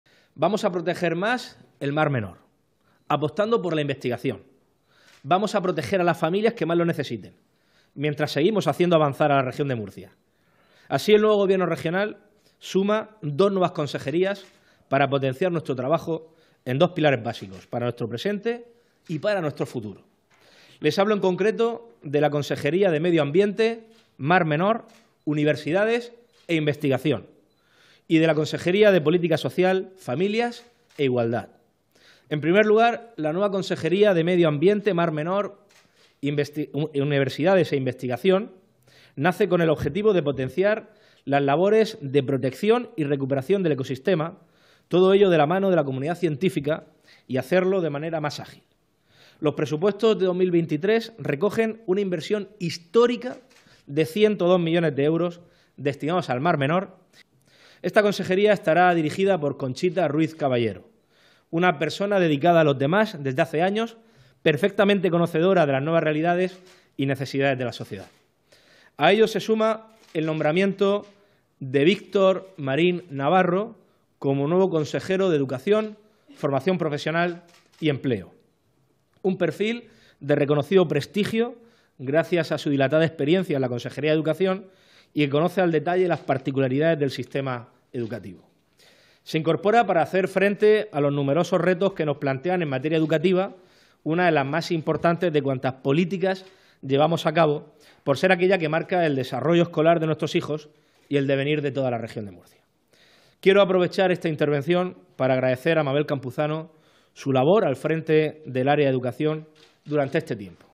López Miras releva a la consejera de Educación, expulsada de Vox, y remodela su Gobierno
El presidente de la Región de Murcia, Fernando López Miras, ha anunciado este lunes la remodelación de su Ejecutivo, a unos meses de las elecciones regionales y tras aprobarse los Presupuestos en la Asamblea, que contempla la creación de dos nuevas consejerías y el relevo de Mabel Campuzano en Educación.

(Fuente: Región de Murcia)